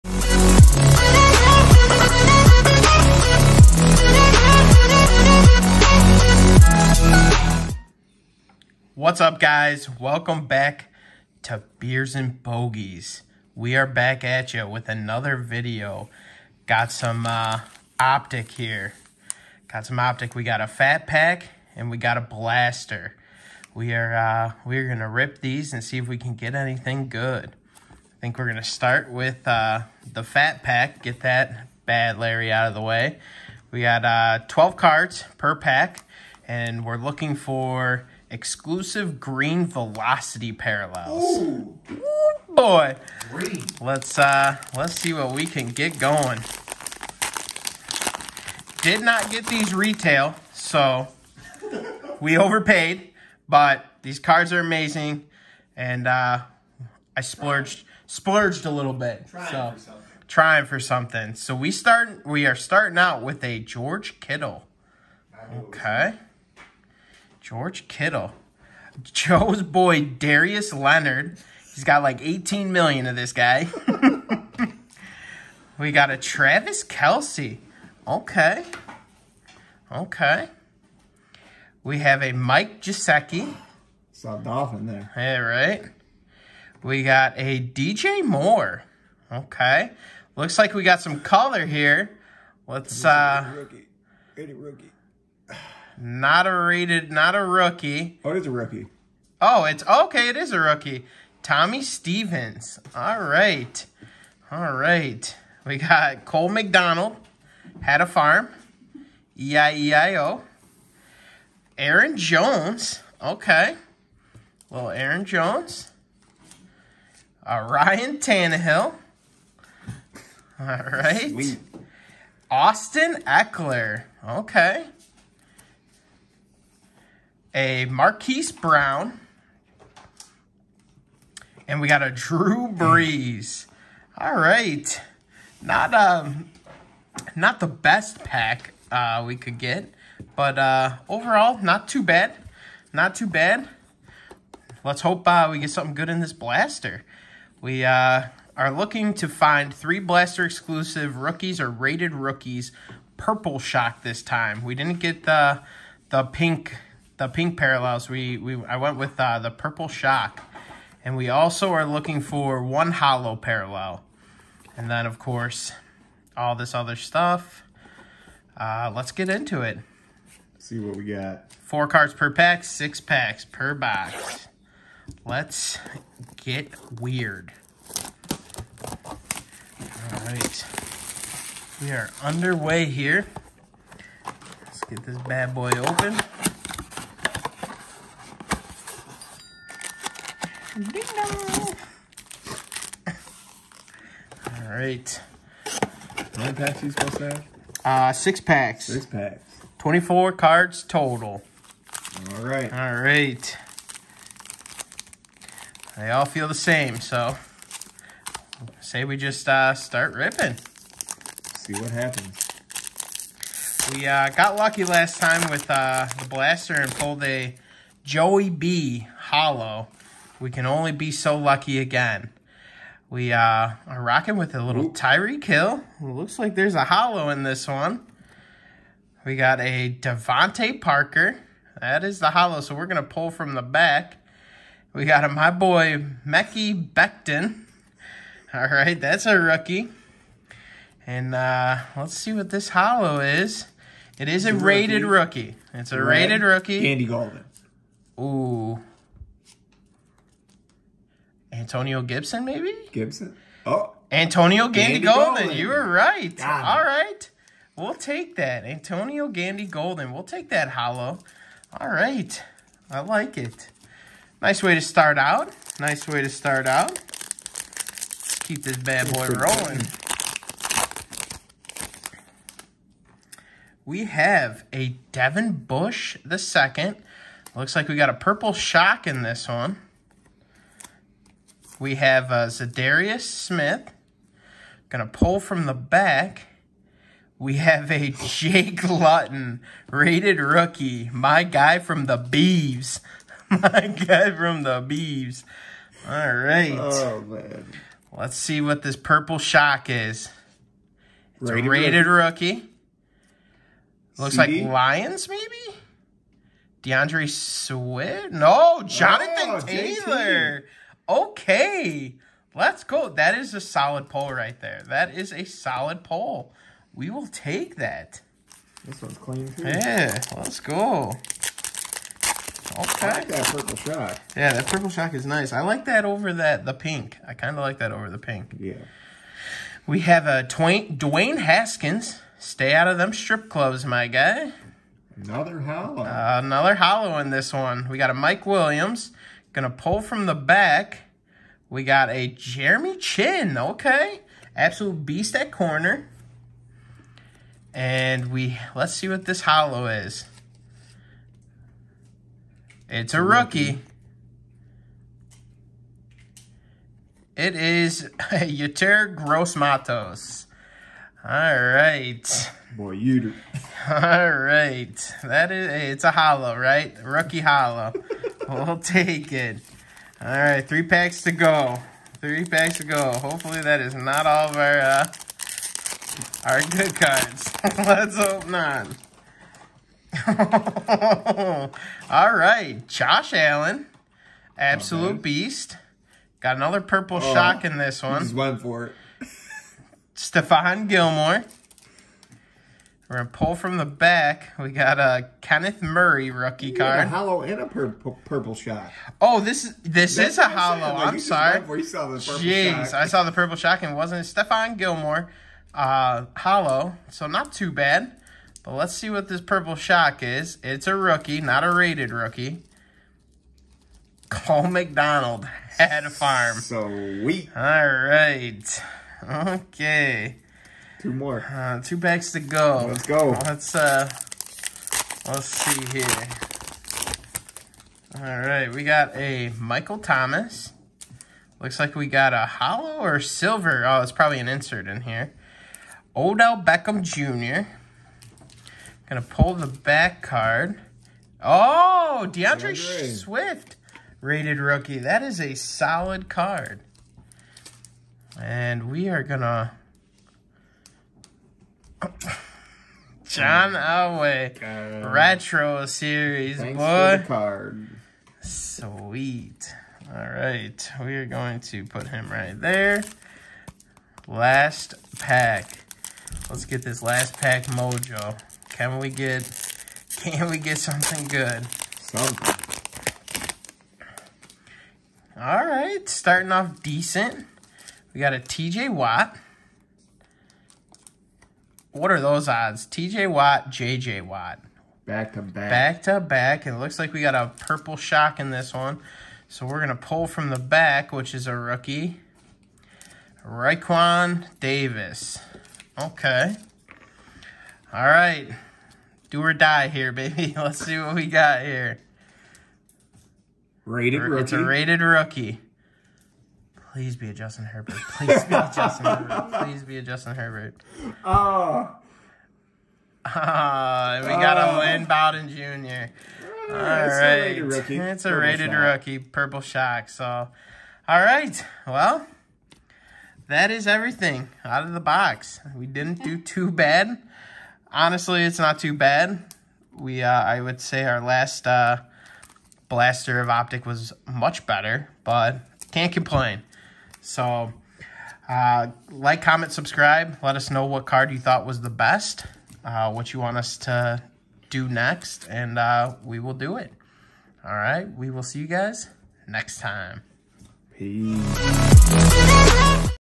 2020 Optic Blaster Box and Cello Pack!
Some more Optic pulls because they've been worth it. Catch us again on Wednesday with an Elite Tmall!